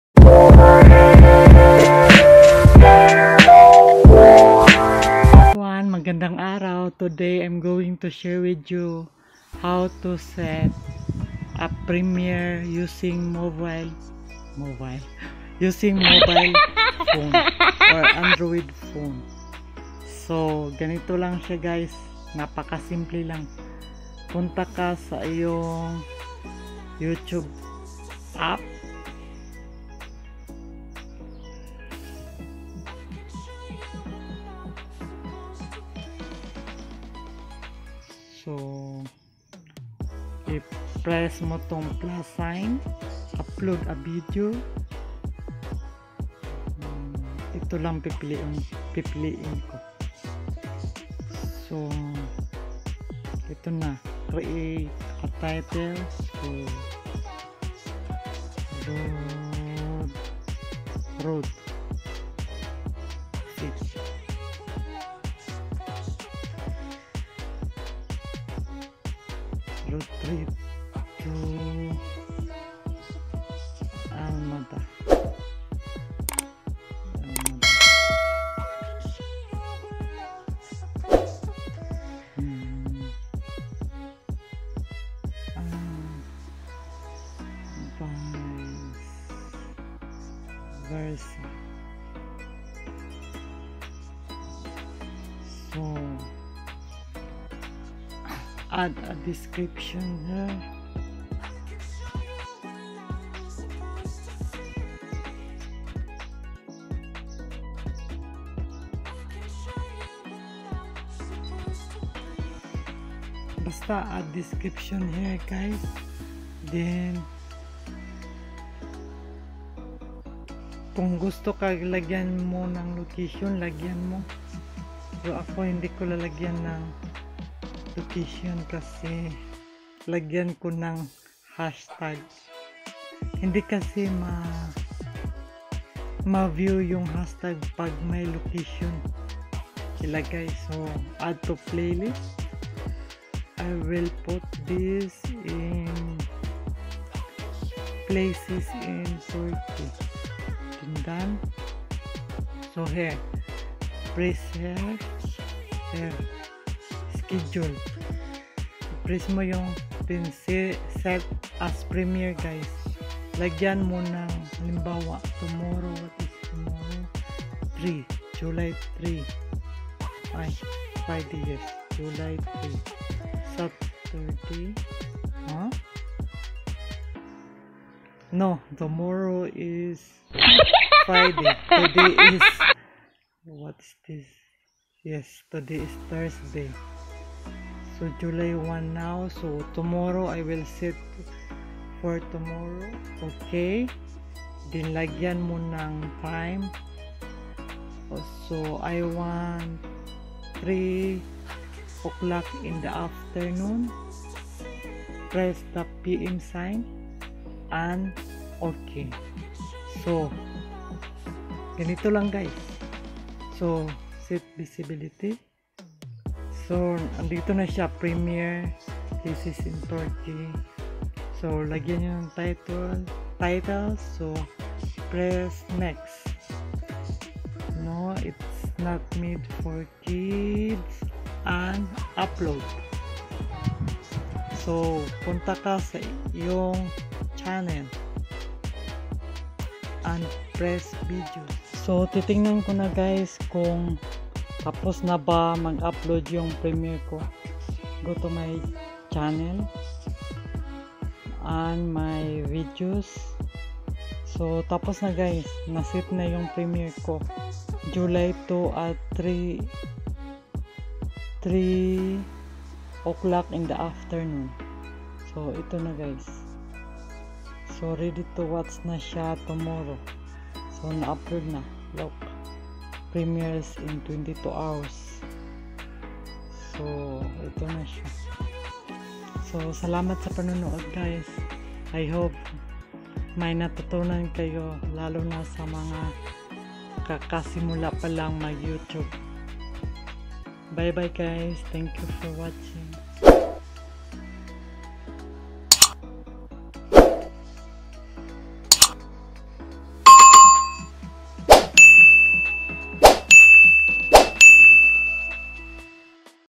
HOW TO SET A PREMIERE VIDEOS ON YouTube USING MOBILE/ANDROID PHONES
In this video I am making a TUTORIAL on How To Set A Premiere Videos on YouTube USING MOBILE/ANDROID PHONES. It will help to those people who just started/don't know how to set a premiere using only their mobile phones. I hope these will help.

I will answer it when I can. Thank you very much. 😘